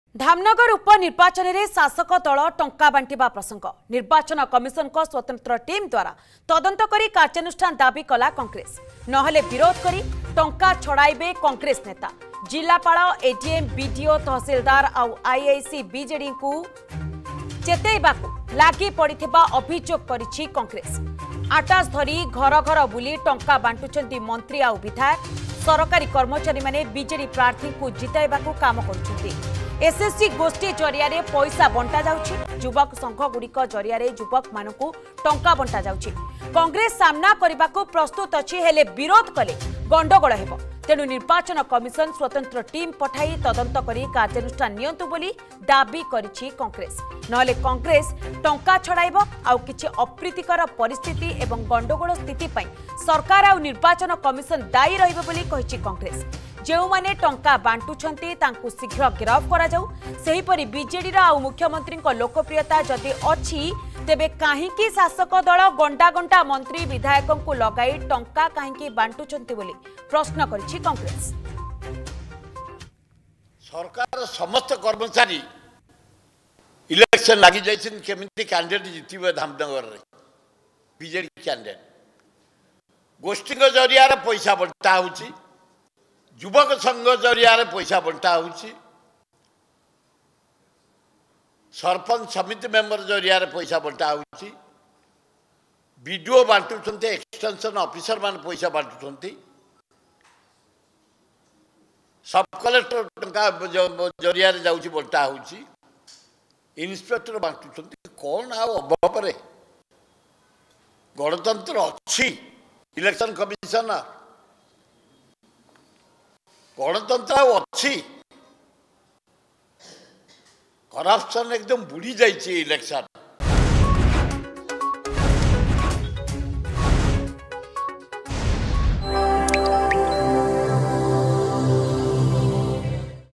ତଦନ୍ତ ଦାବି କଲା କଂଗ୍ରେସ
ଧାମନଗର ଉପନିର୍ବାଚନରେ ଶାସକ ଦଳ ଟଙ୍କା ବାଣ୍ଟିବା ପ୍ରସଙ୍ଗ । ବିଜେଡି ଉପରେ ଖୁବ ବର୍ଷିଲା କଂଗ୍ରେସ । ସ୍ଵତନ୍ତ୍ର ଟିମ୍ ଦ୍ଵାରା ତଦନ୍ତ କରି କାର୍ଯ୍ୟାନୁଷ୍ଠାନ ପାଇଁ ଦାବି । ନହେଲେ ବିରୋଧ କରି ଟଙ୍କା ଛଡ଼ାଇବାକୁ ନେଇ ଦେଲା ଚେତାବନୀ।